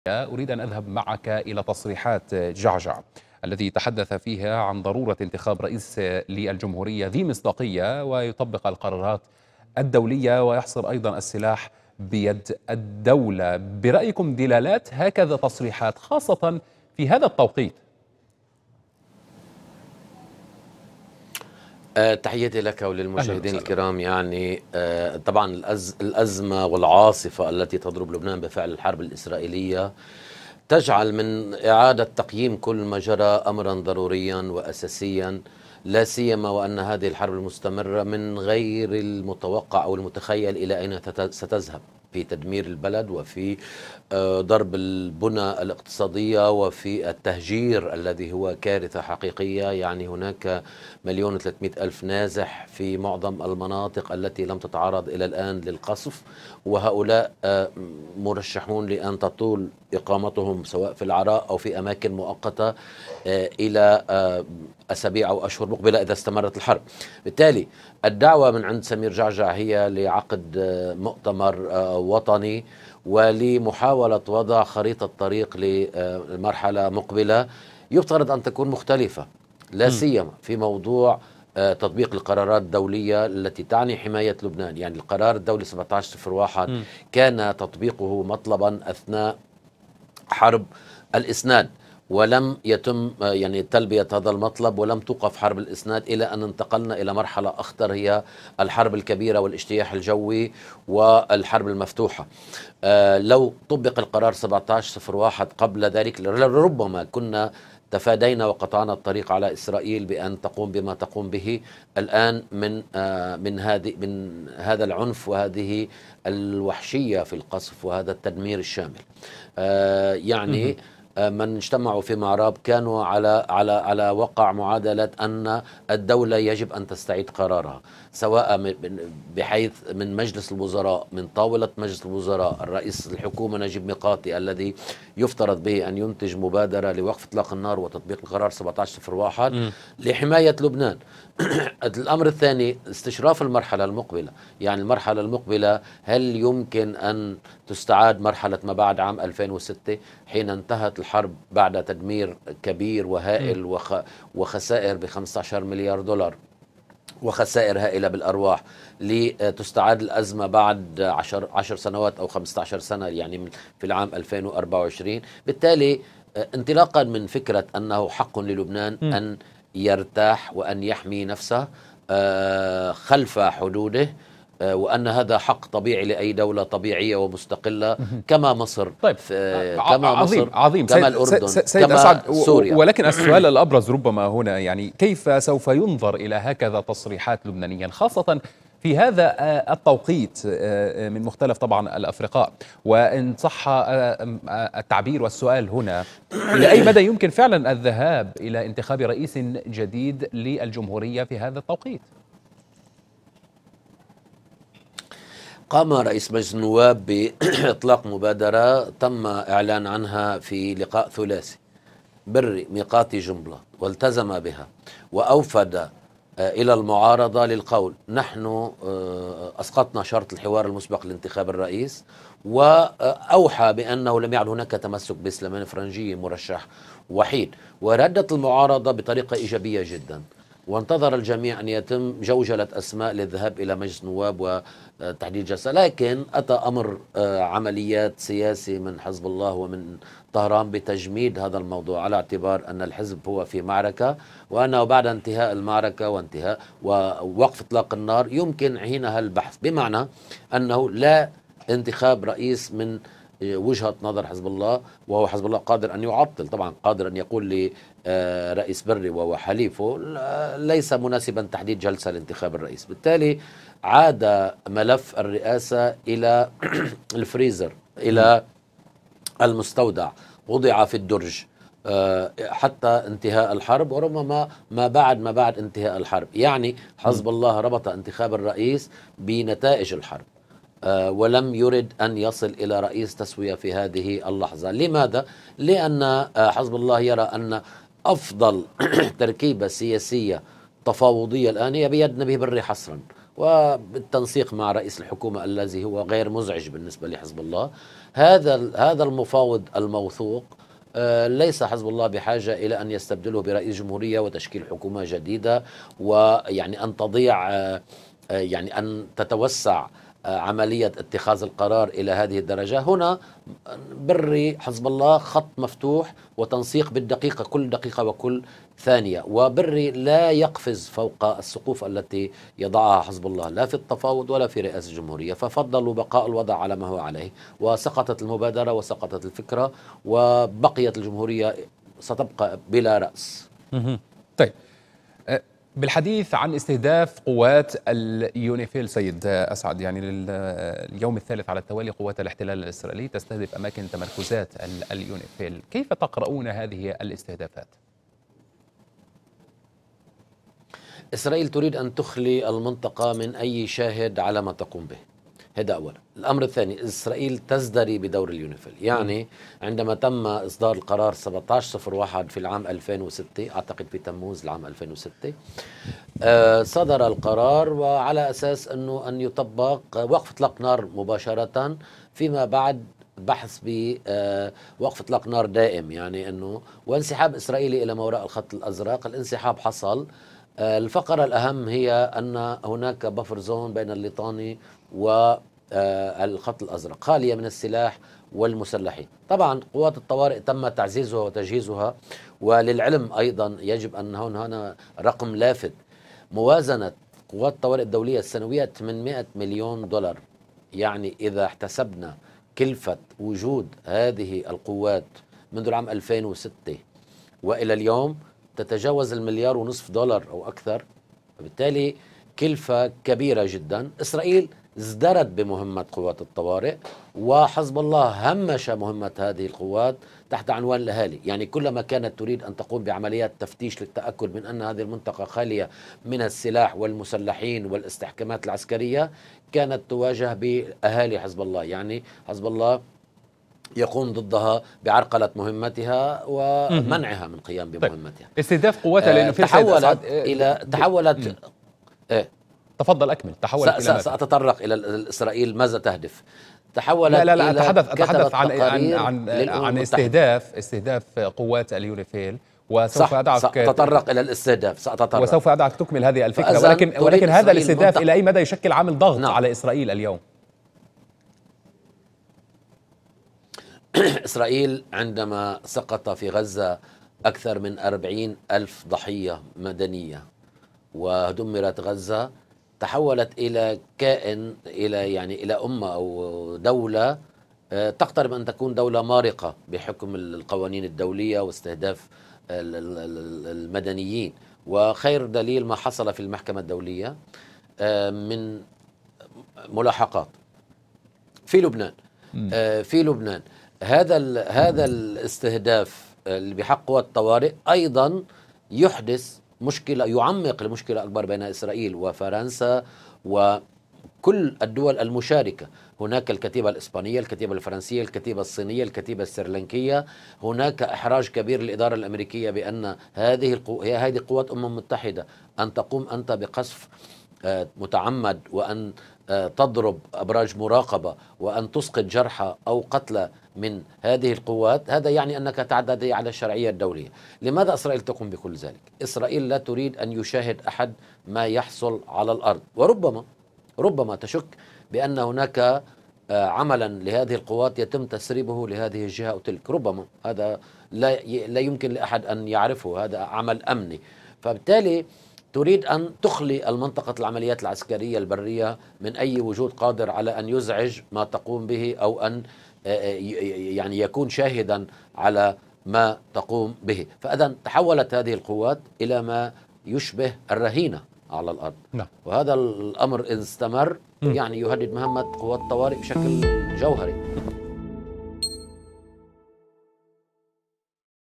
أسعد بشارة: ‏إسرائيل استهدفت قوات اليونيفيل لأنها لا تريد لأحد رؤية ما تفعله على الأرض‏
مستمرون في تغطيتنا من قناة الغد للعدوان الإسرائيلي على قطاع غزة والضفة ولبنان.. لمزيد من التفاصيل ينضم إلينا أسعد ‏بشارة كاتب وباحث سياسي